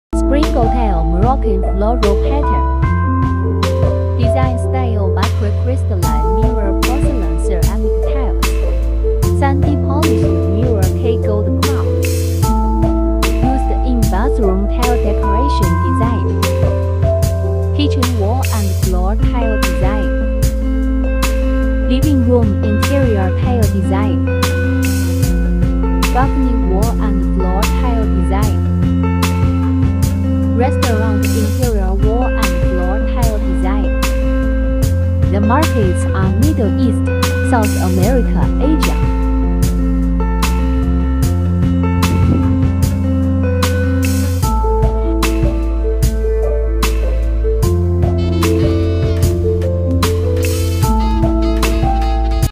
Springletile blue marble tile waterjet interior design
Springletile tile wholesale interior tiles design about Imitation waterjet blue marble tile design, Used in bathroom design and kitchen wall and floor tile design.